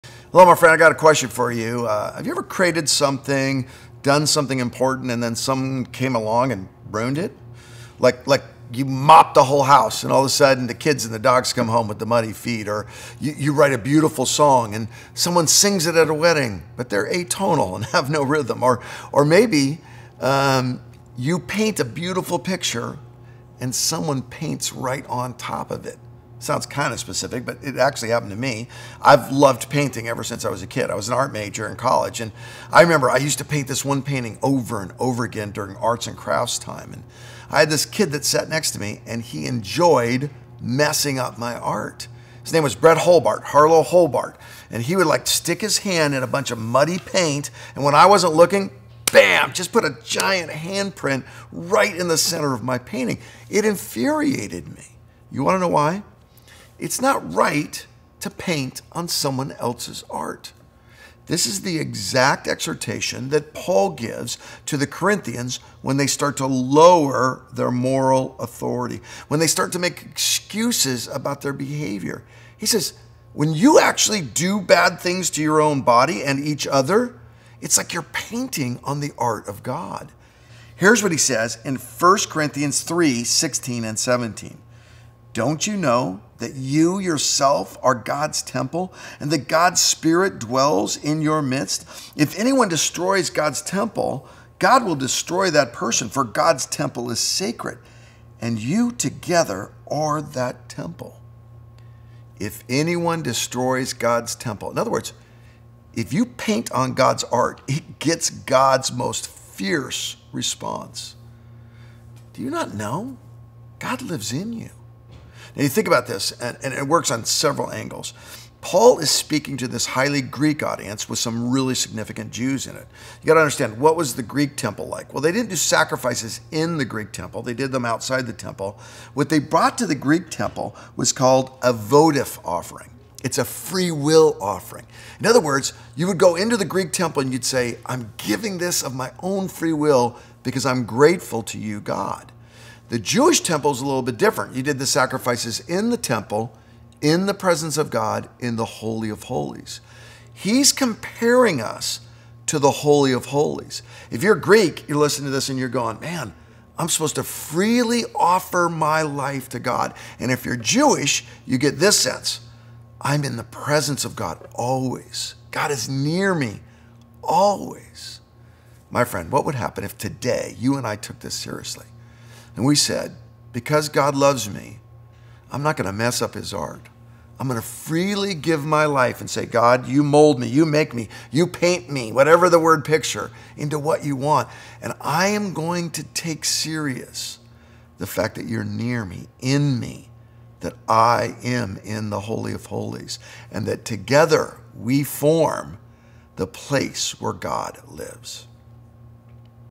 God has created us
We are God’s artwork. In today’s devo, Pastor Curt Harlow asks us what would happen if we allow God to shape us.